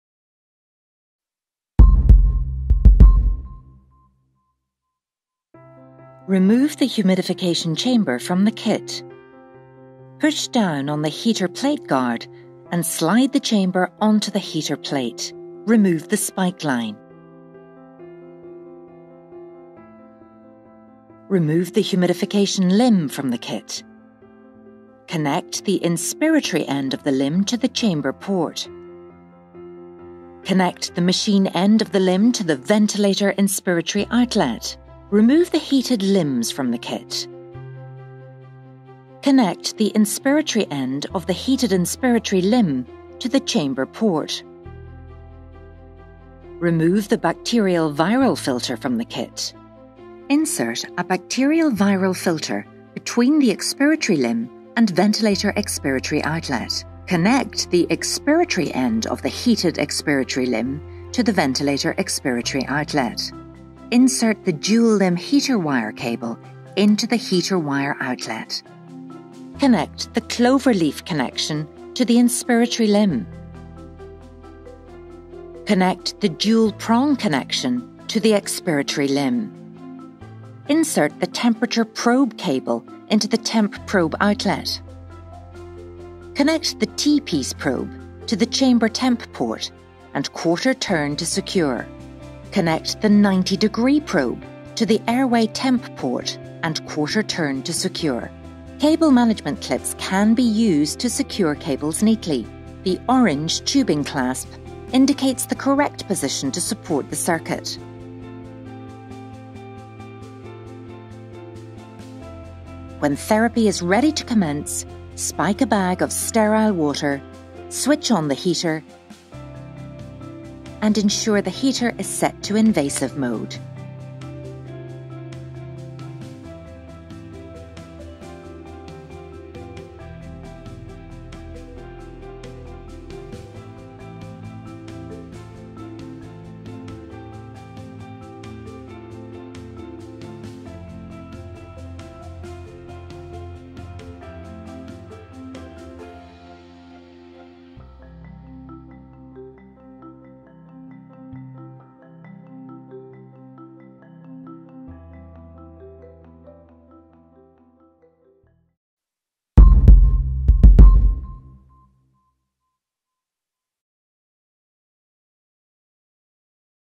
AquaVENT® VT How to use
An advanced ventilator circuit that intuitively manages moisture throughout the system allowing you to focus on the patient.

It is designed to get to the optimum therapy quickly and maintain it consistently.

Optimal humidity is necessary in the management of invasively ventilated patients. With active humidification systems, water in the expiratory limb is a common problem.

AquaVENT® VT intuitively manages moisture throughout the system with a vapour permeable expiratory limb that allows the system to remain dry for the duration of the therapy.